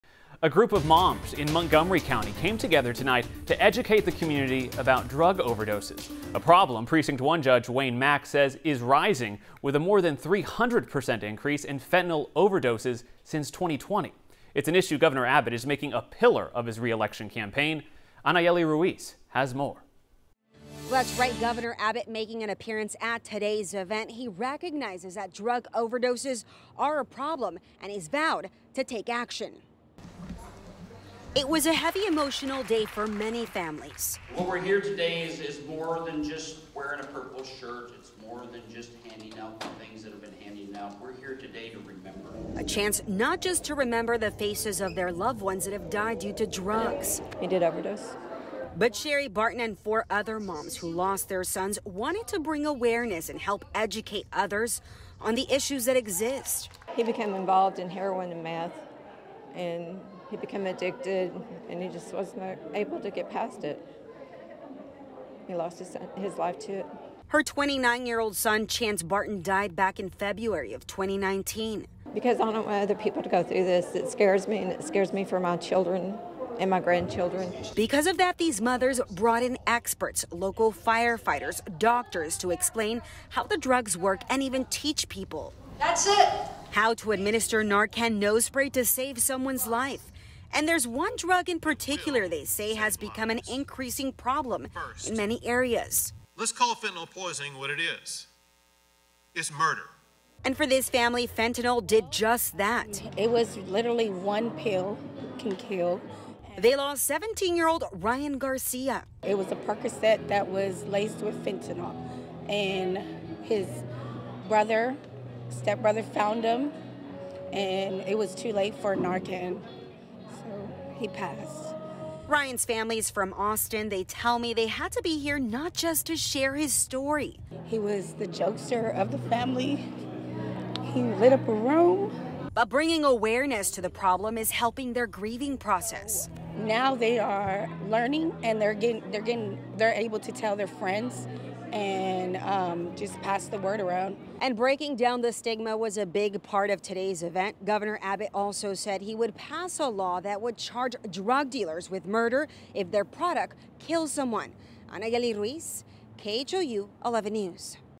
'He overdosed' | Mothers come together in The Woodlands to talk about an increasing problem
A group of moms in Montgomery County came together to educate the community about drug overdoses.